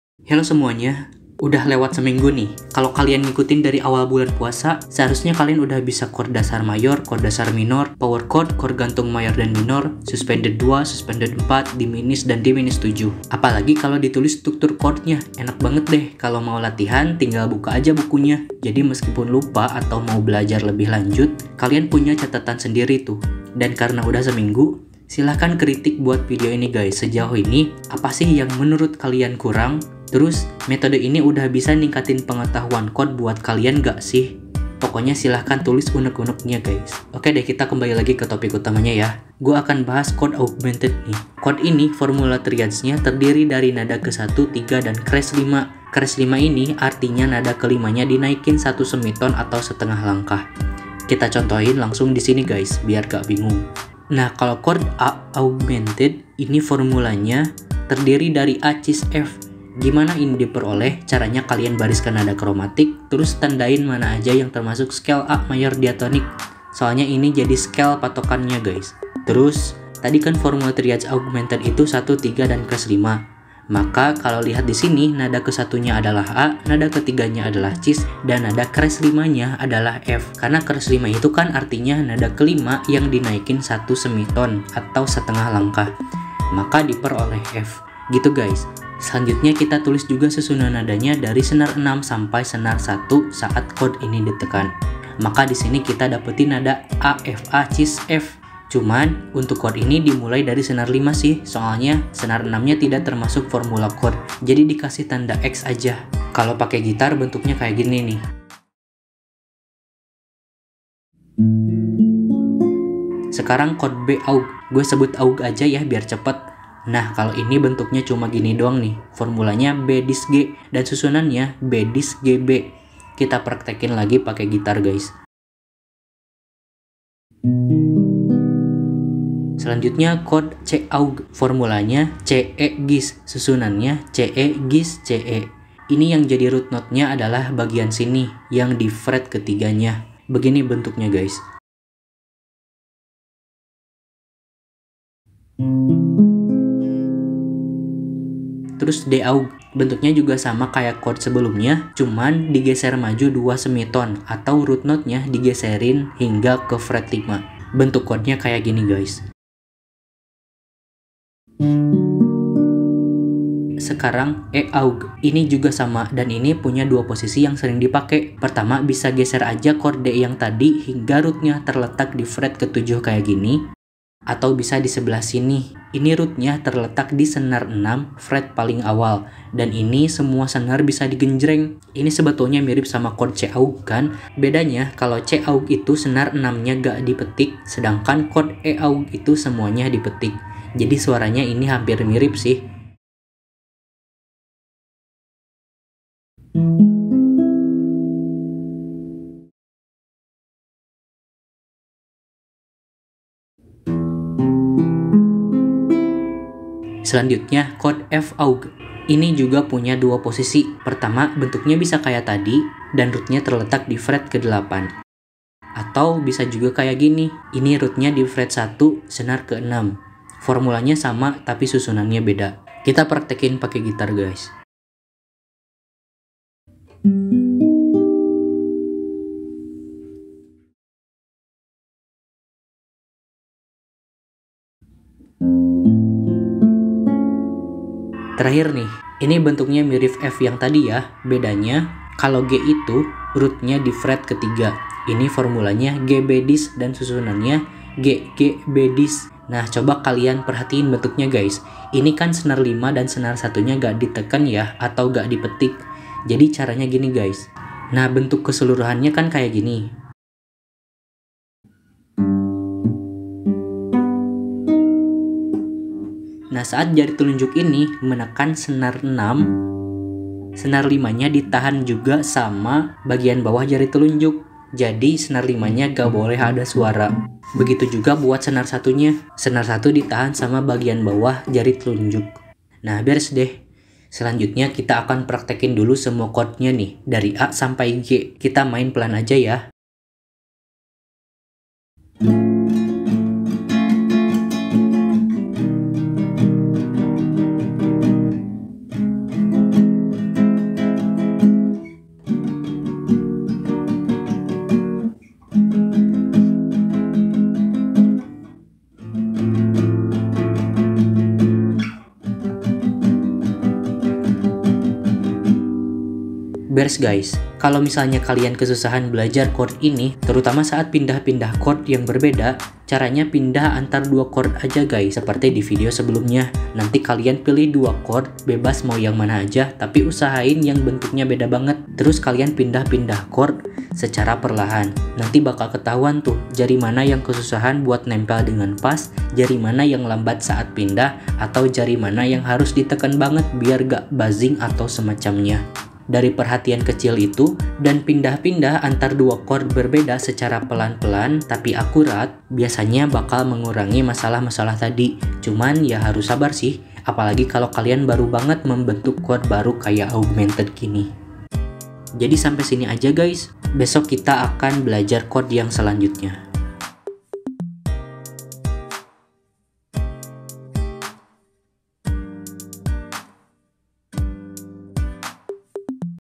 NAMATIN CHORD YG SUARANYA ANEH !? kita tulis dan praktek chord augmented
Namatin semua chord masih berlanjut guys!

Chord augmented ini termasuk chord yg jarang dipake. Jadi, kadang jari tangan gak se-lancar seperti main chord biasanya. Selain itu, bentuknya agak sulit diingat.

Jadi gw bikinin tutorial CARA MENULIS formula chord dan susunan nadanya, sekaligus PRAKTEK pake gitar.